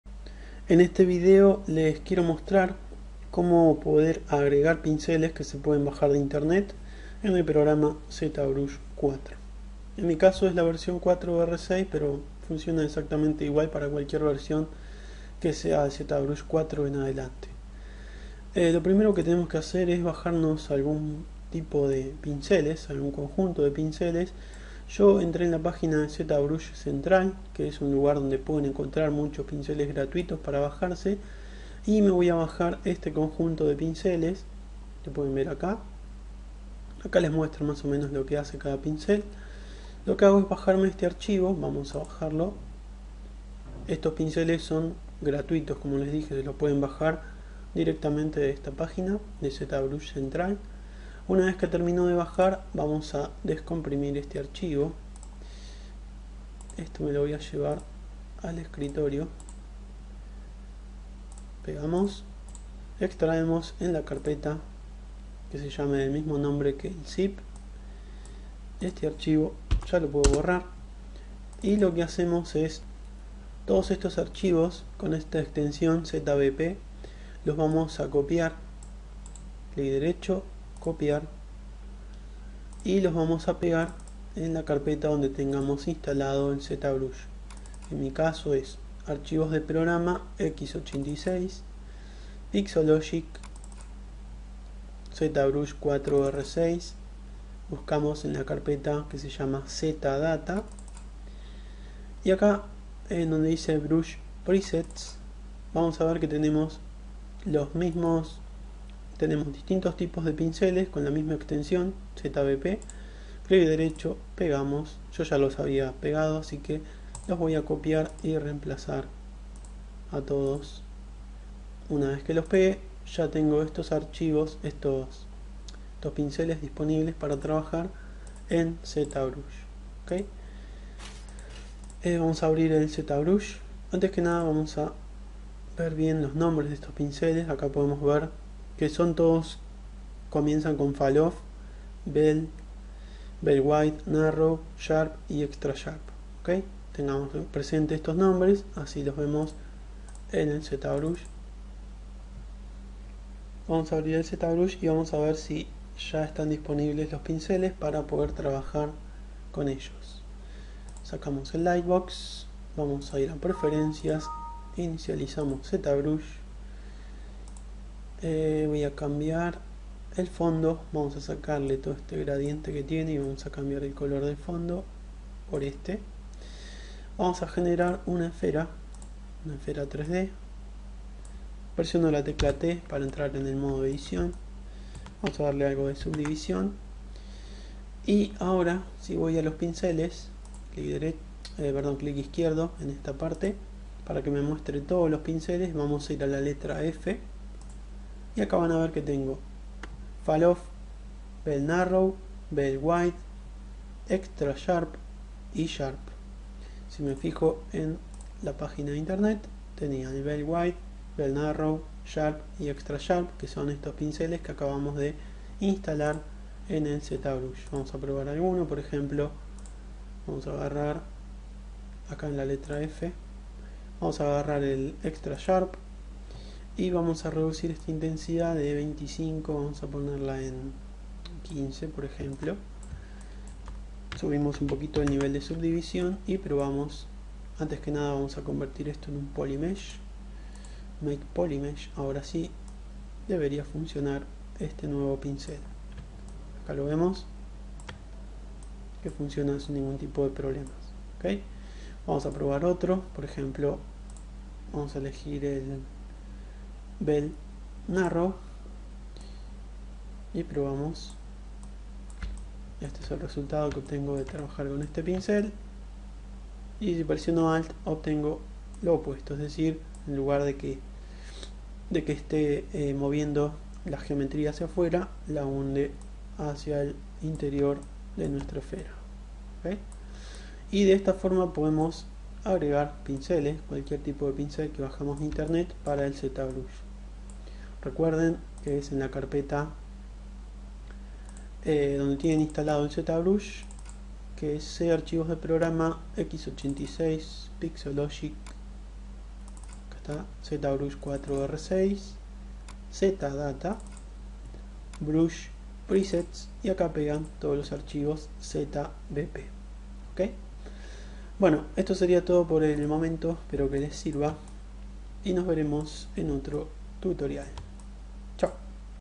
Tutorial Instalar pinceles en Zbrush4
En este video les muestro como poder agregar pinceles que bajamos de internet para poder usarlos en el Zbrush 4R6.